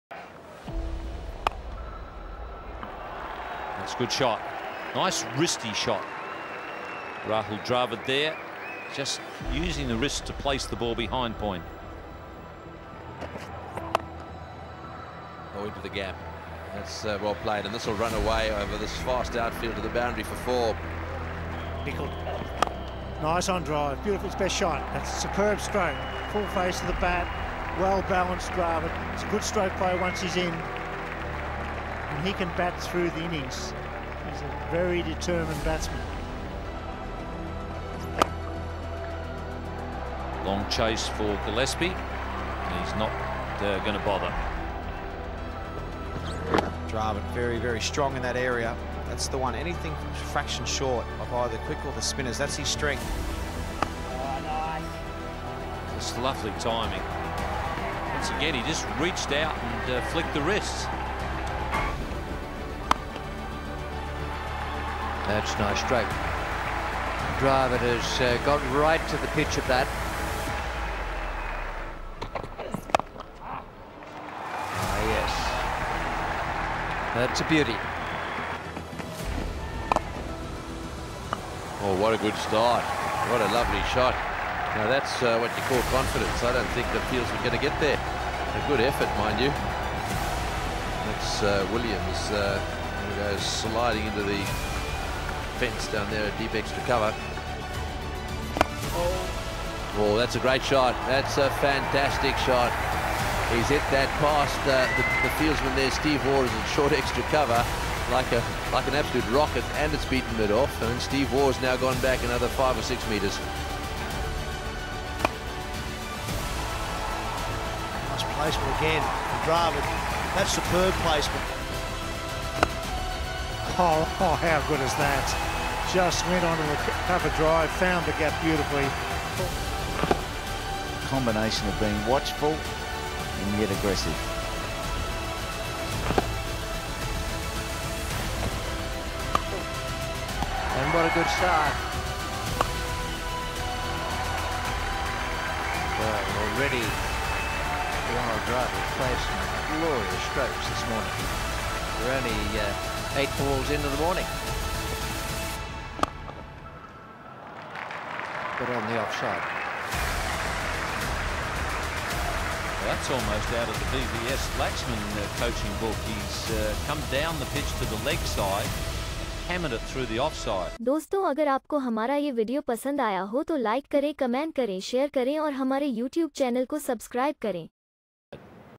strongest Cover Drive of Rahul Dravid
Rahul Dravid’s strongest area of betting - importance of footwork as well hand and eye coordination is also a gift of God